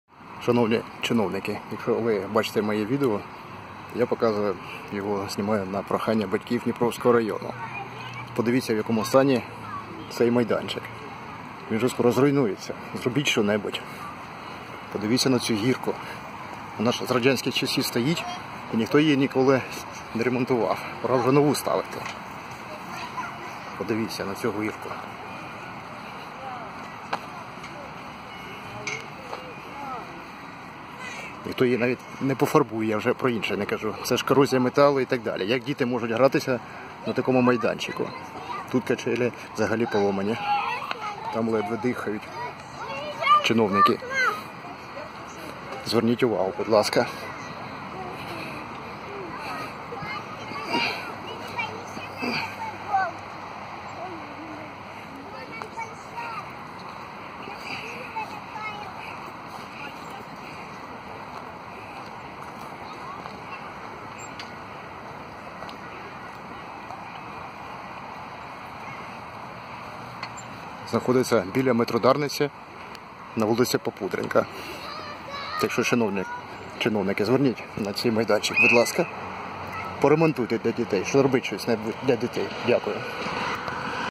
Старий дитячий майданчик біля метро "Дарниця"
Київ. Станція метро "Дарниця". Старий дитячий майданчик